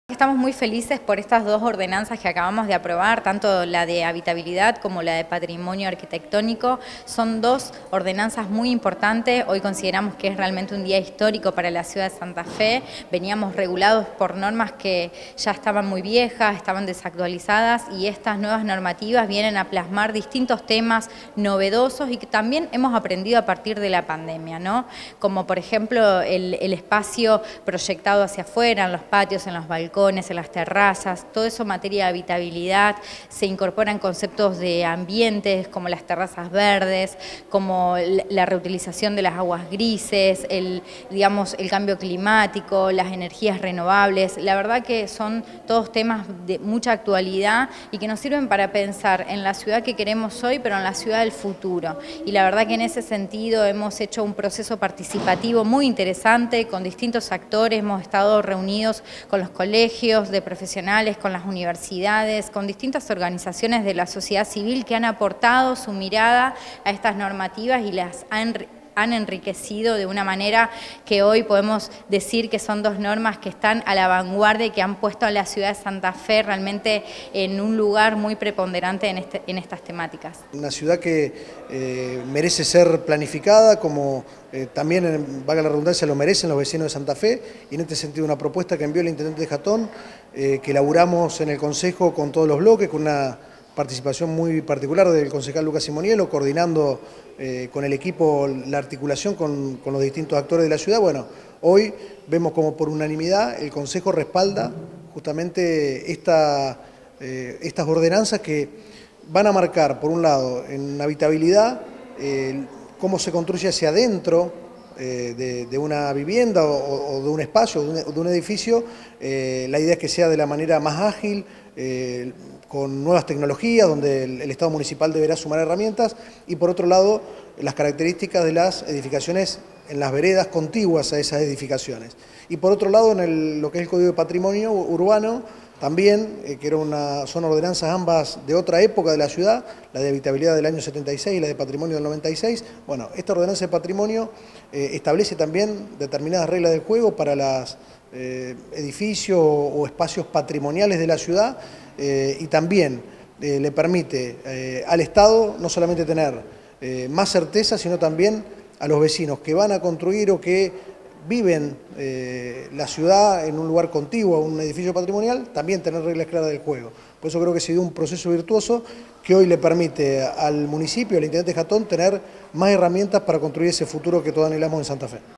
APROBARON EL NUEVO CÓDIGO DE HABITABILIDAD Y PATRIMONIO URBANO
LAURA MONDINO – CONCEJAL

LEANDRO GONZÁLEZ – PRESIDENTE DEL CONCEJO